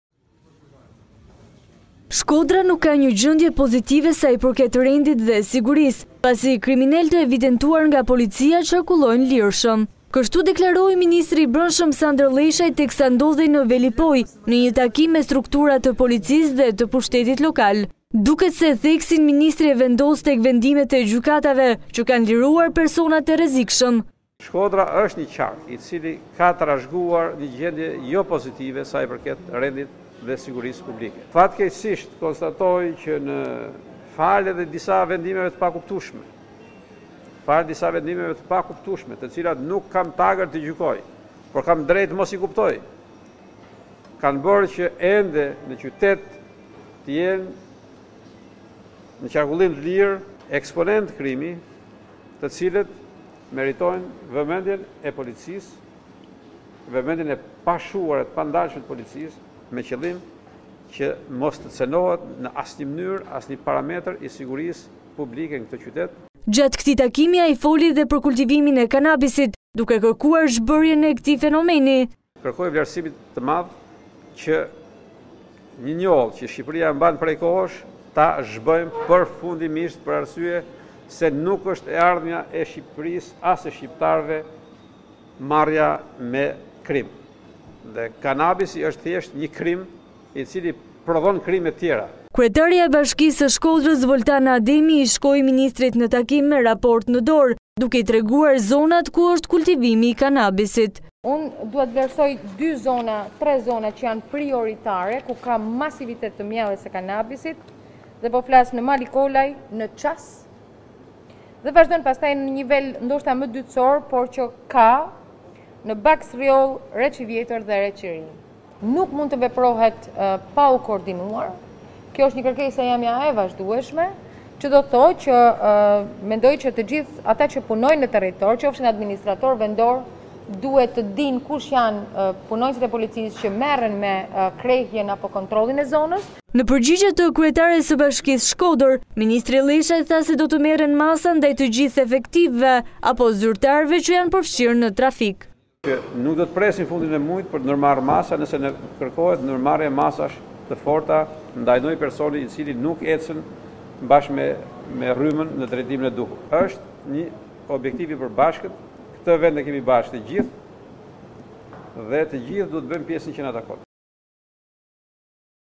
Lleshaj i ashpër nga Shkodra: Nuk i kuptoj disa vendime që nxjerrin të lirë kriminelët!
Turizmi, lufta kundër kanabisit dhe kriminaliteti ishin në fokus të takimit të ministrit të Brendshëm, Sandër Lleshaj me autoritetet vendore në Shkodër. Ministri ka folur me tone të ashpra duke thënë se Shkodra  ka trashëguar një situatë jo të mirë të kriminalitetit.